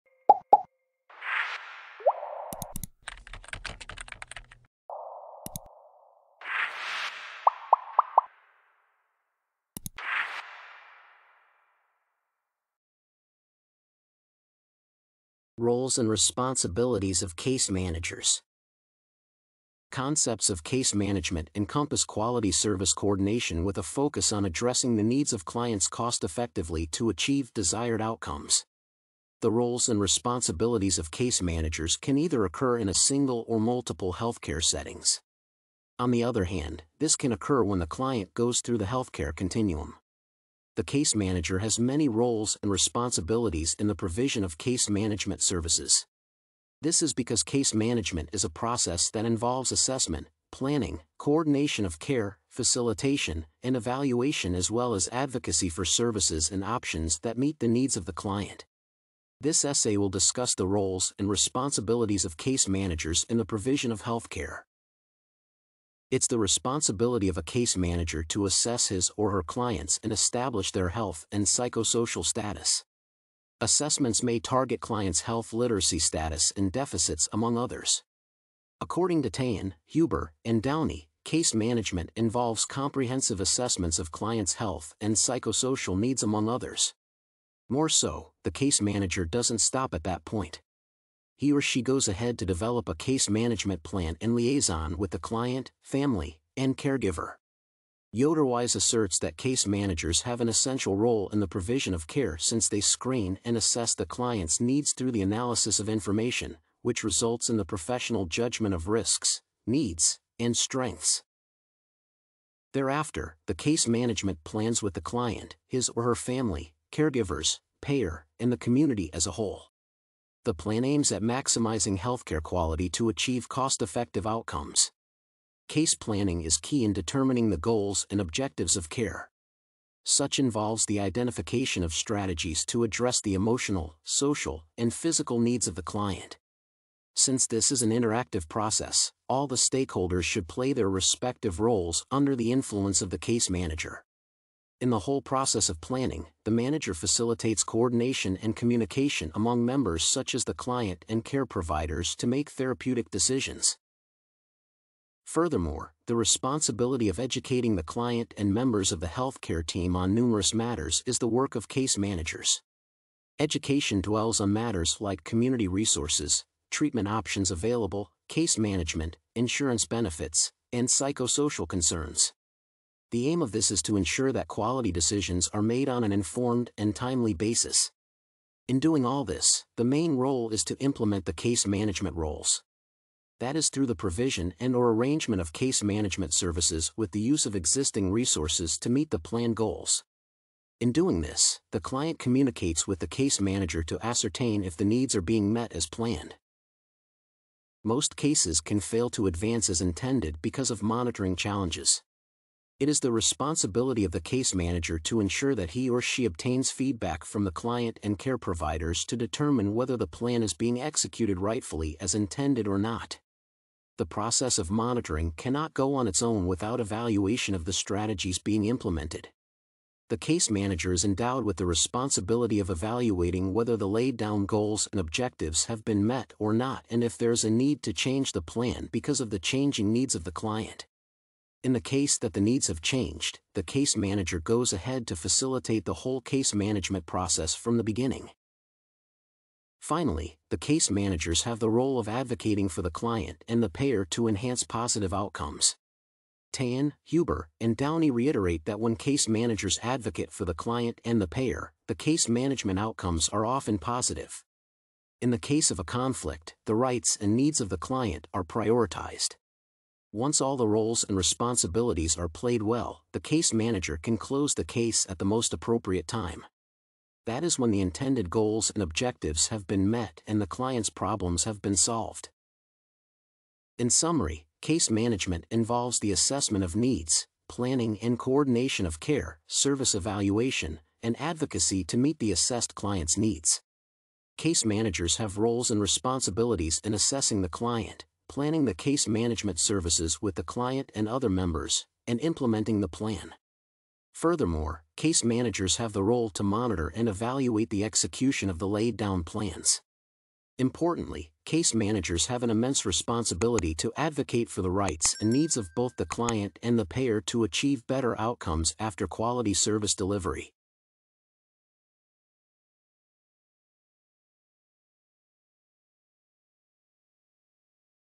Roles and Responsibilities of Case Managers - Essay Example
📝 Essay description: Case management involves the assessment of needs, planning and coordination of care, service evaluation, and advocacy to meet the assessed client’s needs. This essay discusses the roles and responsibilities of case managers in the provision of health care.

ℹ️ Subjects: Administration & Regulation
ℹ️ Words: 808
ℹ️ Pages: 4

📚 Reference list:
Fairman, J. A., Rowe, J. W., Hassmiller, S., & Shalala, D. E. (2011). Broadening the scope of nursing practice. New England Journal of Medicine, 364(3), 193-196.
National Association of Social workers [NASW] (2007). Standards of Practice for Case Management. Washington, DC: NASW Press.
Tahan, H., Huber, D., & Downey, W. (2009). Case Managers’ Roles and Functions: Commission for Case Manager Certification’s 2004 Research, Part I. Lippincott’s Case Management, 11(1), 4-22.
Vanderplasschen, W., Wolf, J., Rapp, R. C., & Broekaert, E. (2010). Effectiveness of different models of case management for substance abusing populations. Journal of Psychoactive Drugs, 39, 81–95.
Yoder-Wise, P. S. (2014). Leading and managing in nursing. Amsterdam: Elsevier Health Sciences.